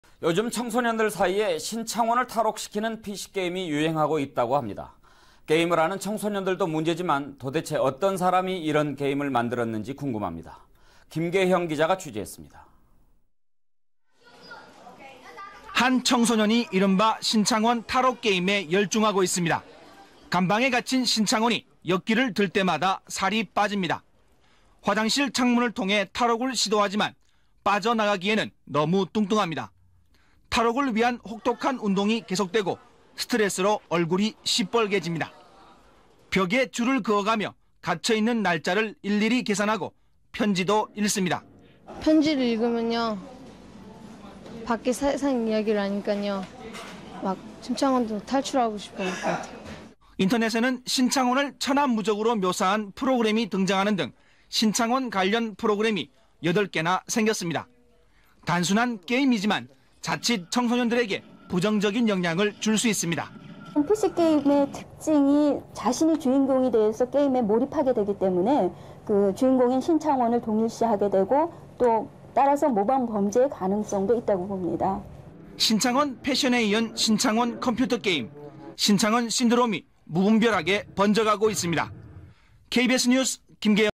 청소년들, 신창원 탈옥시키는 PC게임 유행
김길수 게임도 나올려나